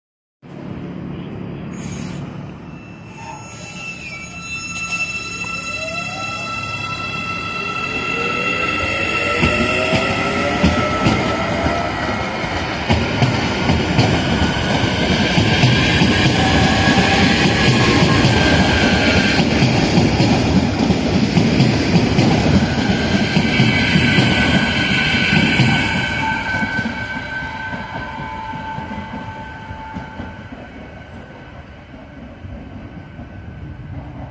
Linha 710 da CPTM SP 95
Tue Hyundai Rotem série 9500
Partindo da estação perus sentido Jundiaí
Filmado na linha 710 da CPTM